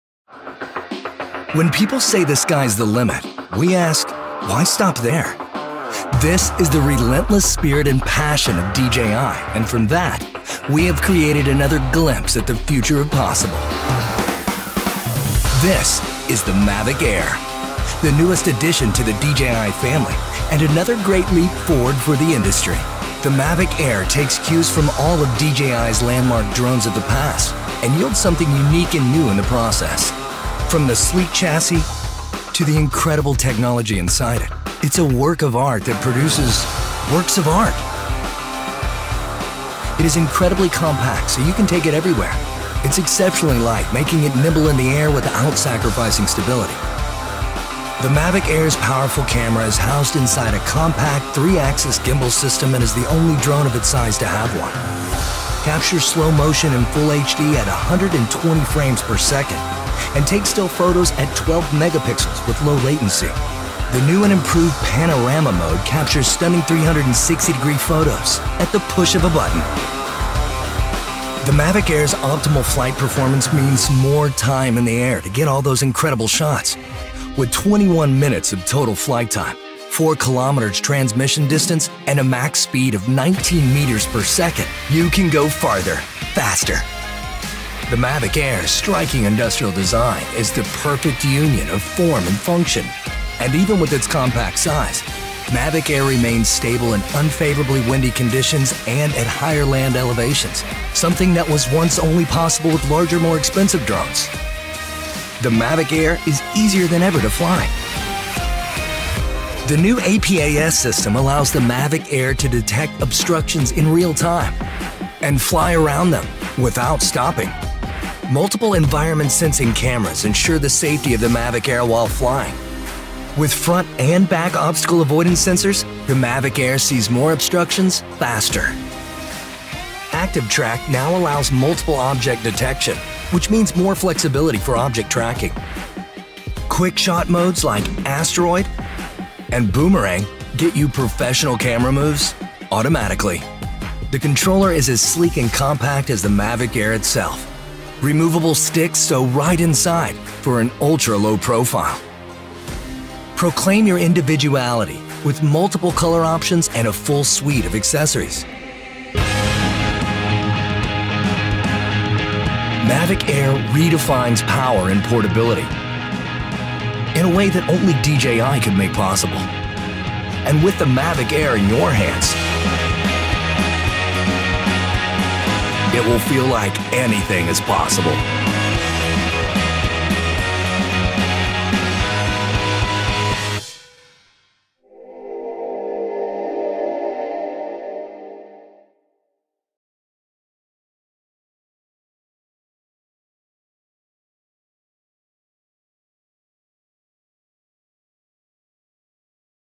Der neue DJI Mavic Air, Loockup
Ein Wunderwerk der Technik und des Designs, das Mavic Air wurde gebaut, um überall dort hin zu gehen, wo Abenteuer Sie hinführt. Diese ultraportable und faltbare Drohne ist die beste der Mavic-Serie und bietet High-End-Flugperformance und -Funktionalität für grenzenloses Erkunden.

A marvel of engineering and design, the Mavic Air was built to go wherever adventure takes you. Inheriting the best of the Mavic Series, this ultraportable and foldable drone features high-end flight performance and functionality for limitless exploration.